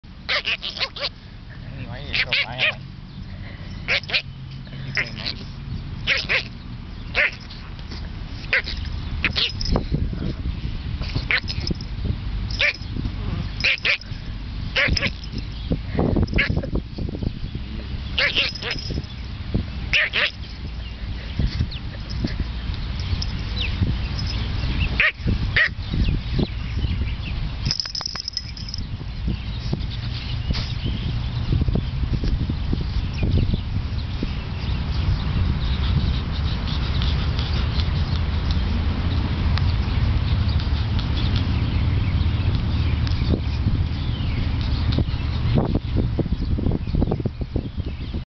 Face slapping Chihuahua?
It looks like Mimi is slapping Zeke's face left and right.  Poor Zeke!